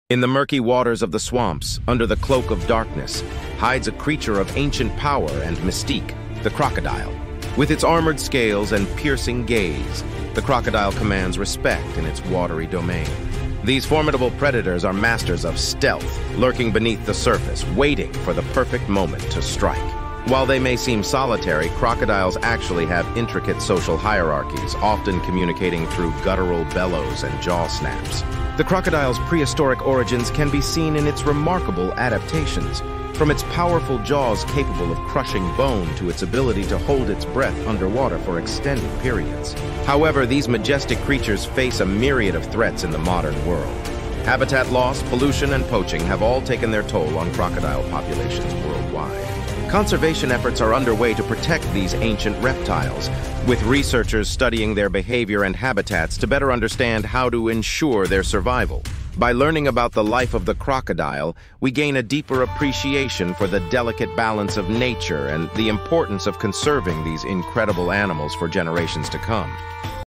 Crocodile Chronicles: Secrets of the Swamp
Explore the hidden world of crocodiles and discover the challenges they face in the modern age. Join us on a journey into the heart of the swamp as we unravel the mysteries of these ancient predators.